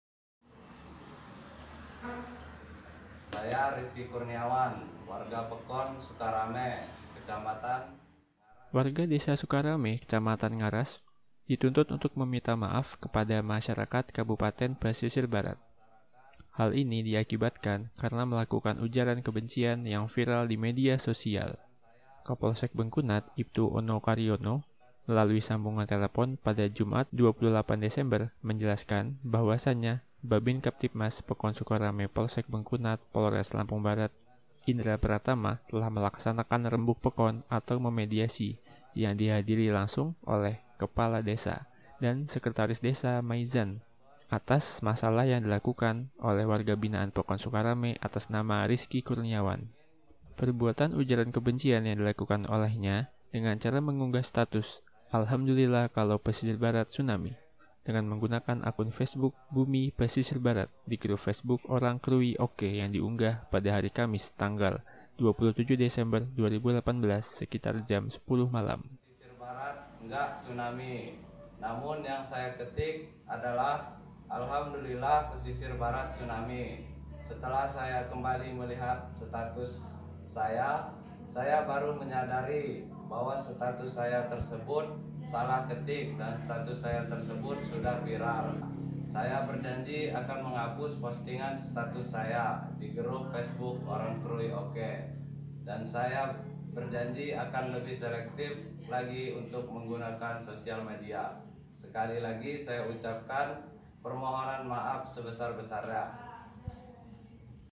Lagi, Ujaran Kebencian Tentang Tsunami Melalui Medsos, Pria Ini Minta Maaf | KUPASTV LAMPUNG
Sebarkan Ujaran Kebencian Di Medsos, Warga Pesibar Memohon Maaf

Rizki Kurniawan warga Pekon (desa) Sukarame Kecamatan Ngaras, dituntut untuk meminta maaf kepada masyarakat Kabupaten Pesisir Barat (Pesibar).

Kapolsek Bengkunat, Iptu Ono Karyono, melalui sambungan telpon (Jumat 28/12), menjelaskan bahwasannya Bhabinkamtibmas Pekon Sukarame Polsek Bengkunat Polres Lampung Barat Bripda Indra Pratama telah melaksanakan Rembug Pekon atau memediasi yang dihadiri langsung oleh peratin (kepala desa) Sahidi dan Sekretaris Desa Mazyan Pekon Sukarame Kec. Ngaras permasalahan yg dilakukan oleh warga Binaan Pekon Sukarame atas bama Rizki Kurniawan.